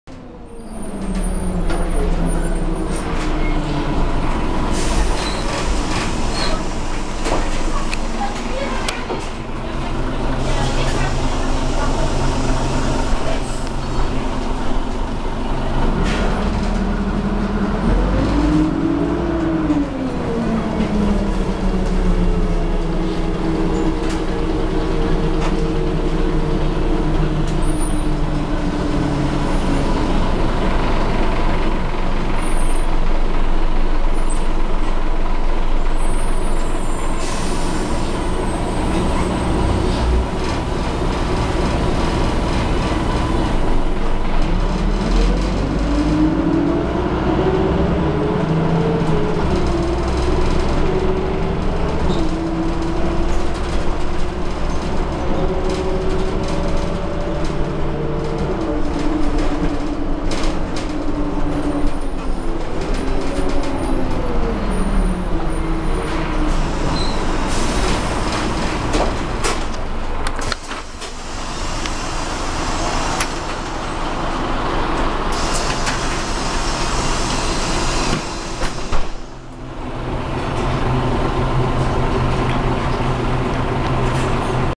[Orléans] Roadmovie R312 - TAO
Roadmovie en R312 à Orléans, sur la ligne 2 du réseau TAO.